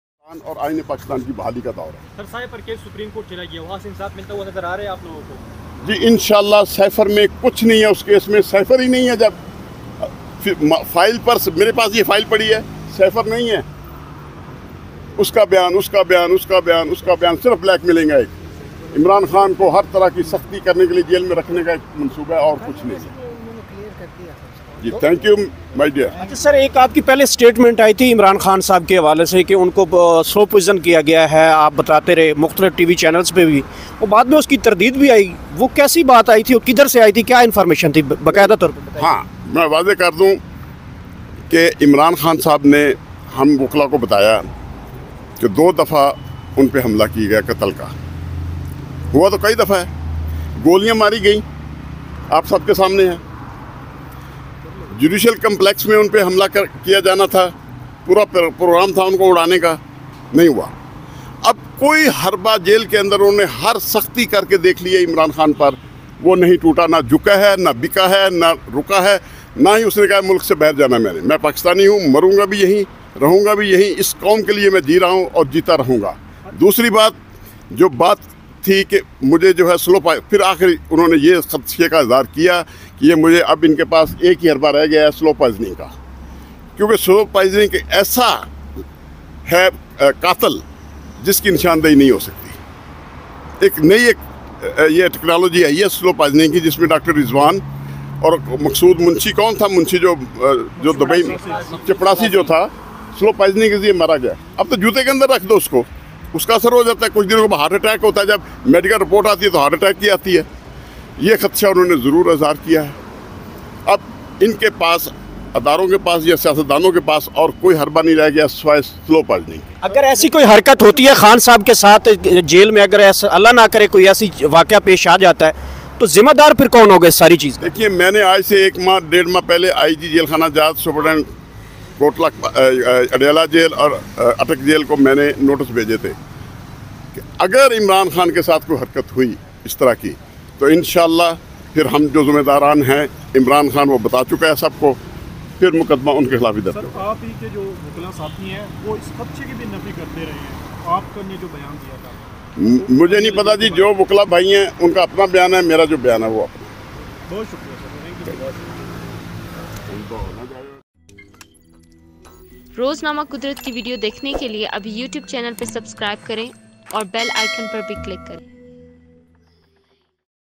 🔴 Is the Cipher Case Completely Over? Imran Khan's Good News! 📰☔
سائفر کیس مکمل ختم ؟عمران خان سرخرو ؟وکیل آفتاب باجوہ عدالت سے نکلتے ہی برس پڑے
In this latest QudratNews update, we dive deep into the ongoing Cipher Case involving Imran Khan. Is there good news on the horizon, or is it a twist in the tale? Watch the video to find out more. Plus, we'll discuss Lawyer Aftab Bajwa and the unexpected rain shower that followed his court appearance. 🔔📺